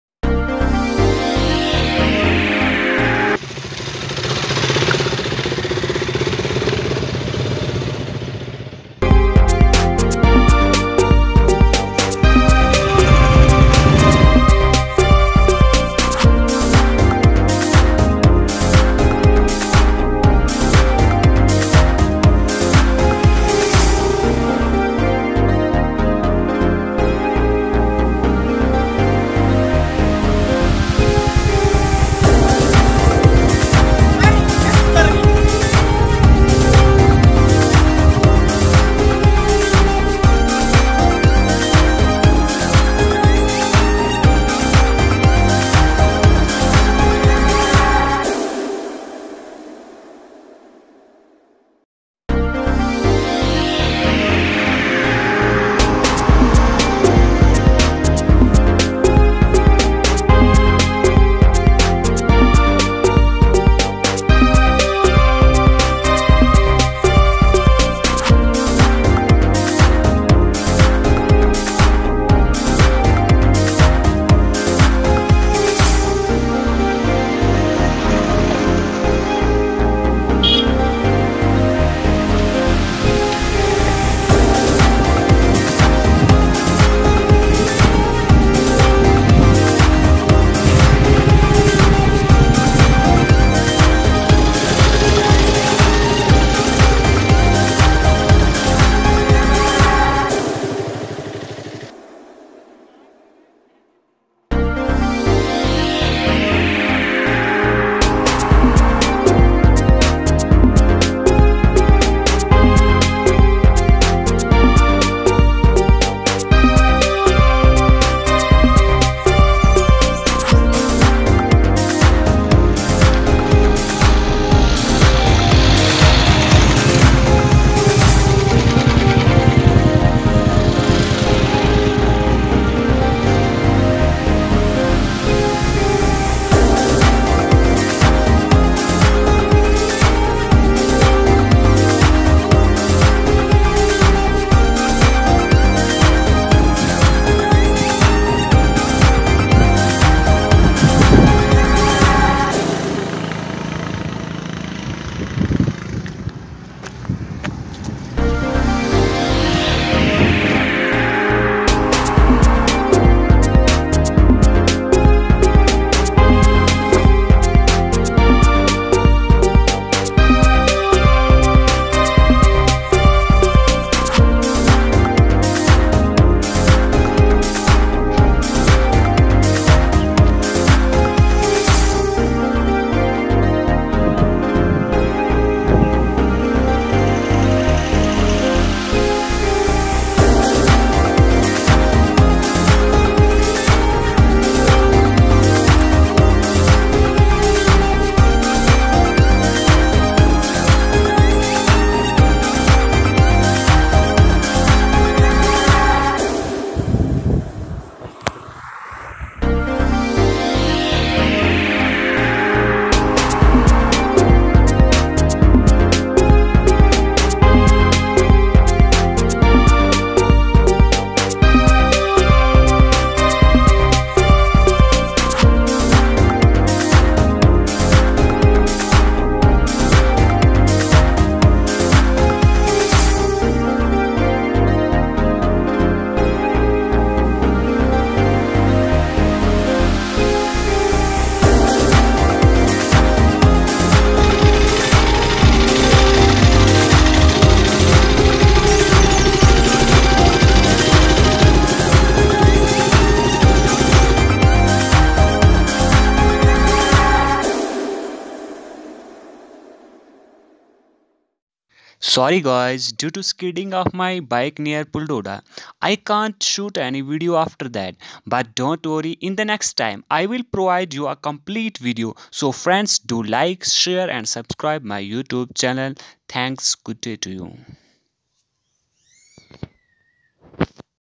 BIKE RIDING ( jammu to doda )
My first experience of long ride from jammu  to doda...
one of the best experience of my life.
And my dream also fulfilled. Hardly matters if someone difficulties occur during journey. 💪💪💪